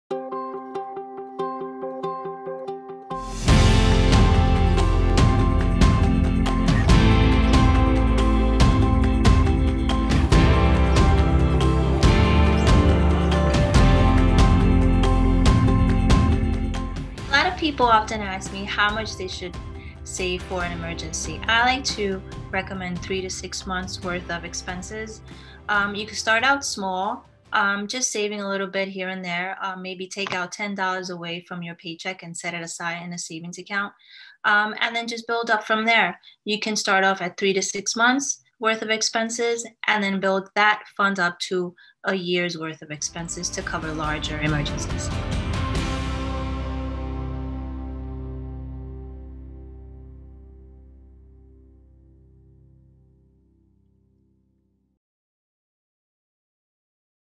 Real​ Money, Real Answers: How much should I have in emergency savings?
Ana Gonzalez Ribeiro, AFC® shares her advice on being prepared. Tune into our YouTube channel every other Monday for more great tips from AFC® professionals! RealMoneyRealAnswers​ AskanAFC​